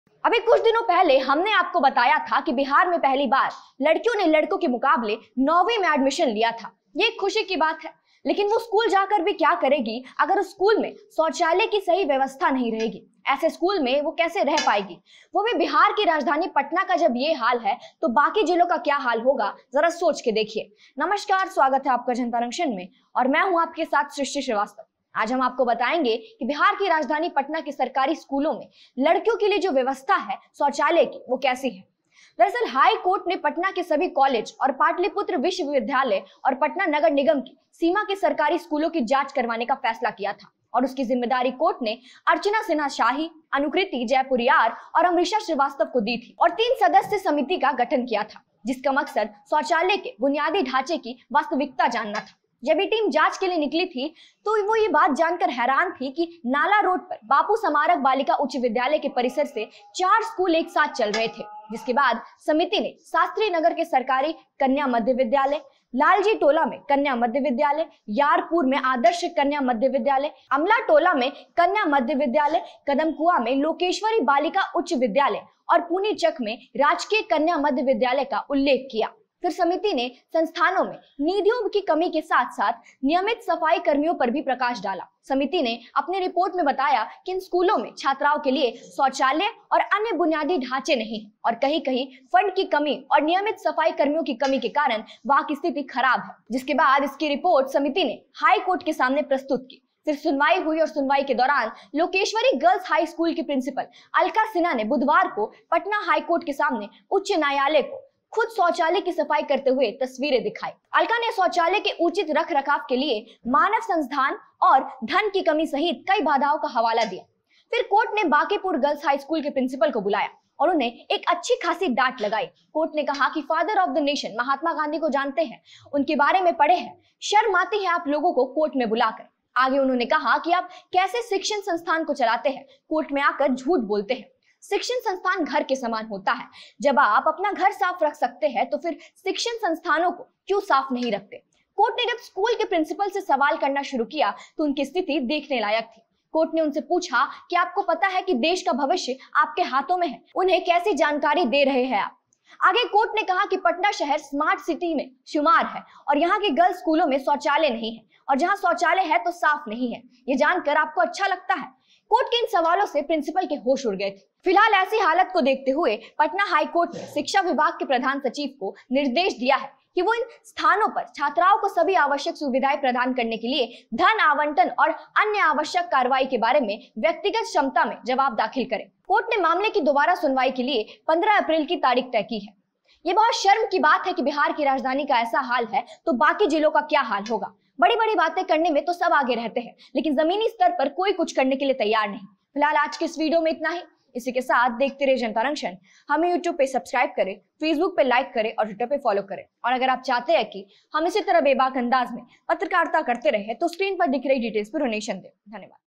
Govt. School में लड़कियों के लिए शौचालय नहीं होने से Patna Highcourt हैरान, जमकर लगाई फटकार
सरकारी स्कूलों में लड़कियों के लिए शौचालय नहीं होने से हाईकोर्ट ने नाराजगी जताते हुए प्रिसिंपल को जमकर फटकार लगाई है. देखिए ये रिपोर्ट:-